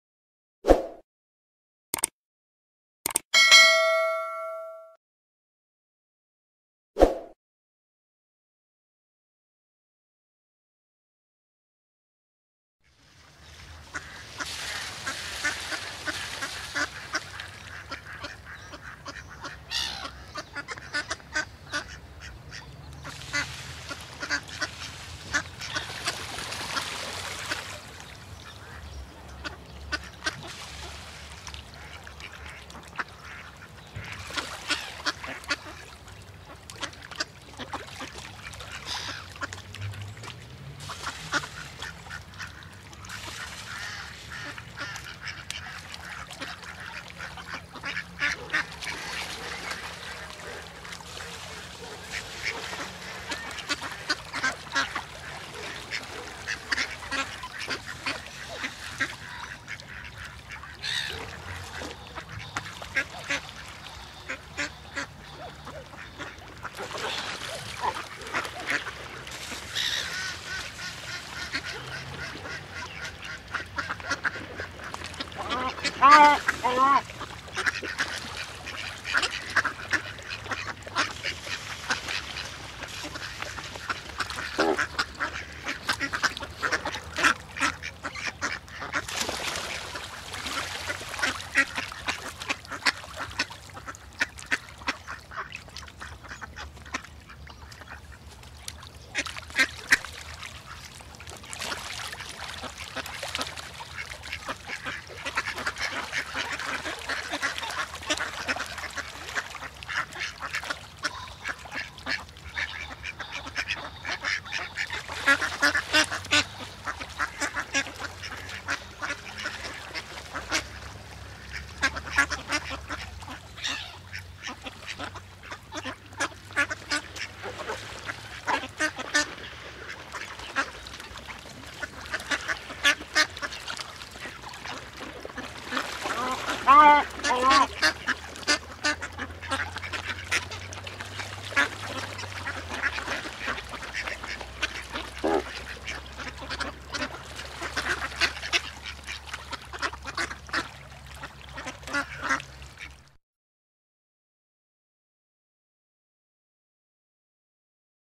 great white pelican & duck Swimming in the water/ Rubel shaikh 0.2,
great white pelican & duck Swimming in the water

It's nice to see a lot of ducks swimming together and it's nice when they sound together

rubel shaikh 0.2#

amazon products=

flipkart products=